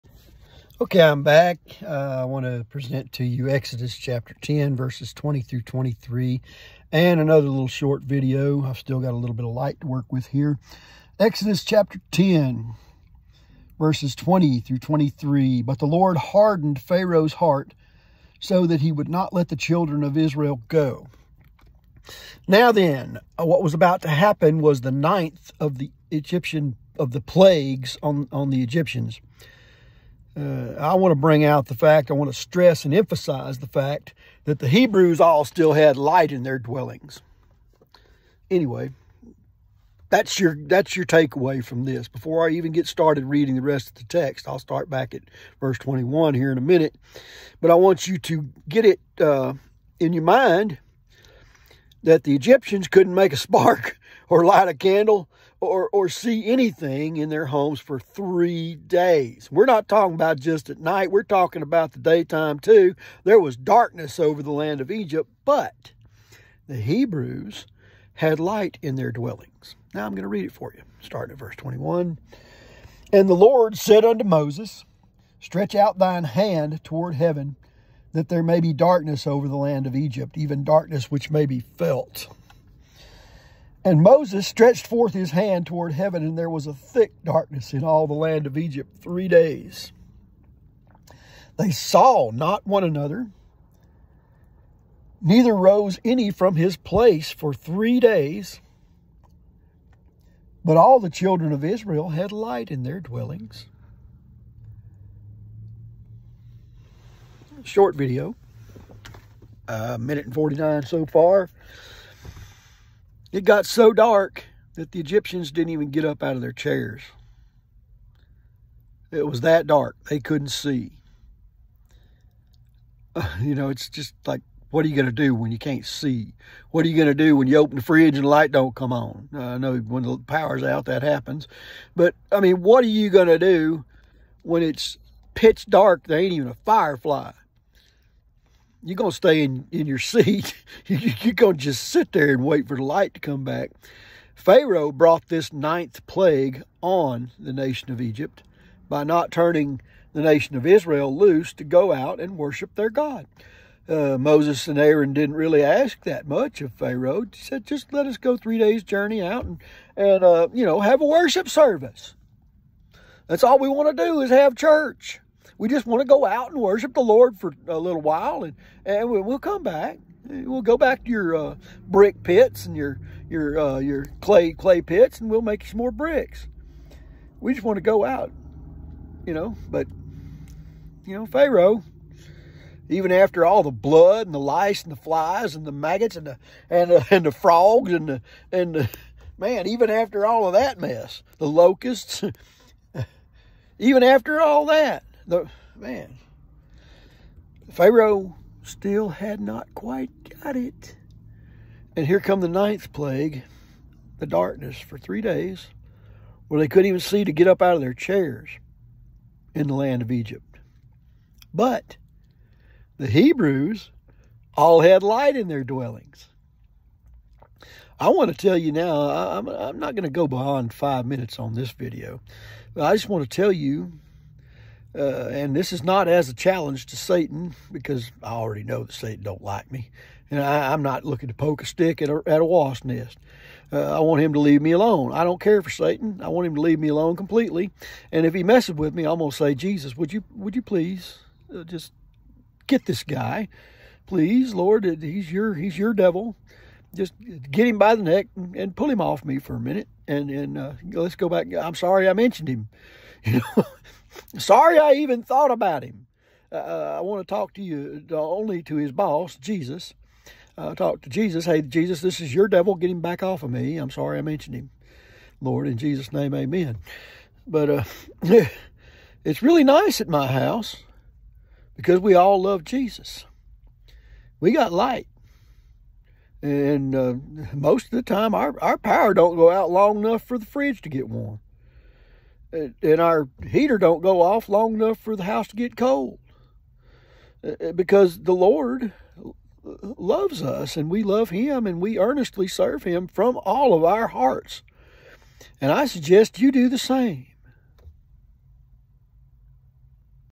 Video Three from 09/13/2024: Exodus 10:20-23
I stopped in Spring Lake Park here in Texarkana on the afternoon of September 13th, 2024 to make some videos with my phone. The first two videos, which were longer than this one, were taken from  Deuteronomy 28:14-68.
In this video, I talk more about the difference between being blessed and being cursed, and my text herein will be taken from Exodus 10:20-23.